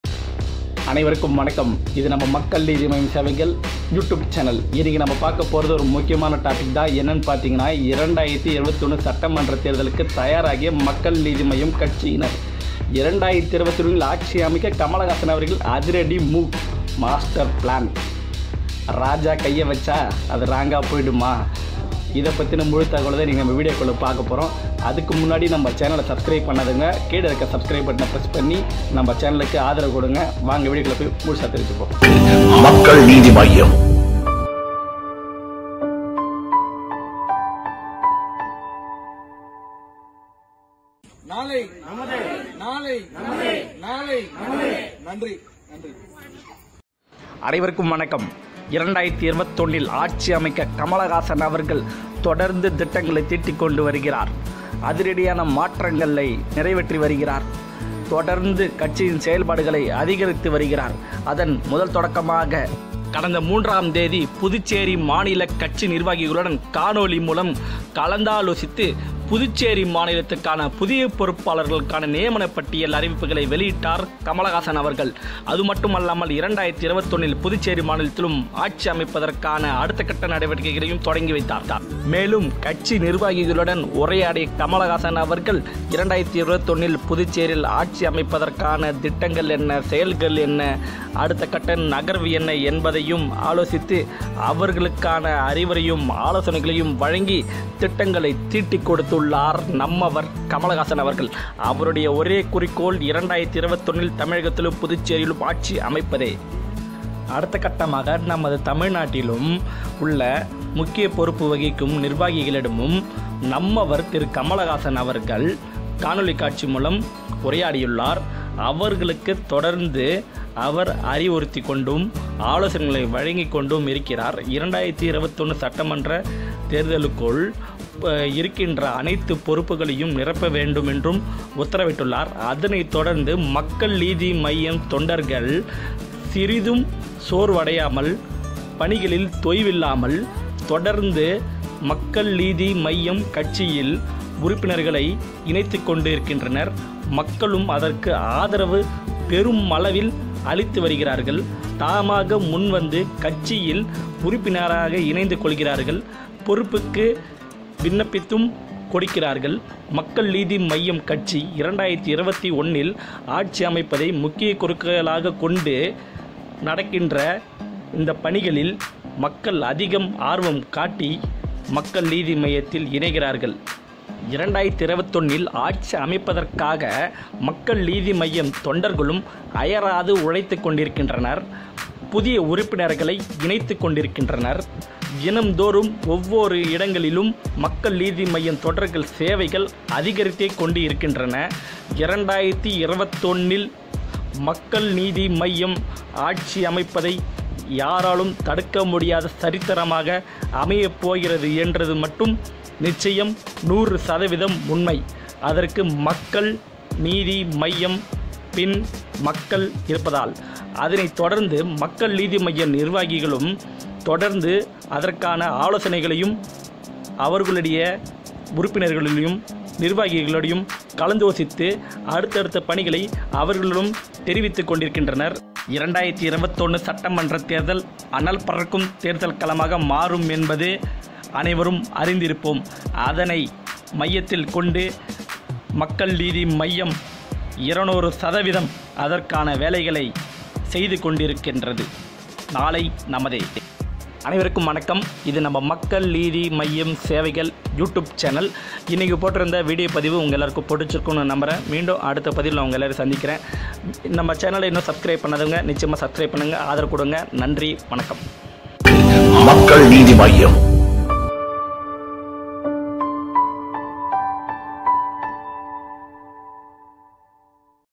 Makkal Needhi Maiam Latest News
கமல்ஹாசன் அவர்களின் அதிரடி திட்டங்கள்! 2021 ஆட்சி அமைப்பதில் தீவிரம் !